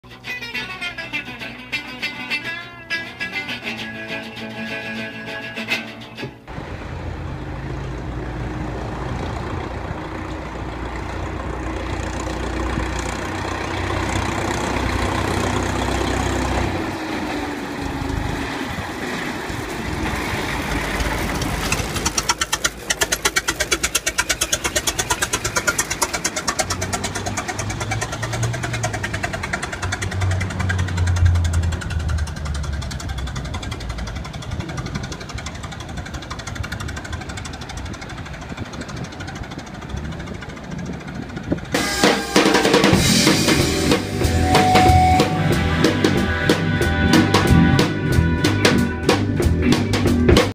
Jay Leno's 1911 Christie Fire Engine
Summer of 2018. Jay Leno rolling up Sunland Blvd on his 1911 Christie Fire Engine. Segment by Richard Alan White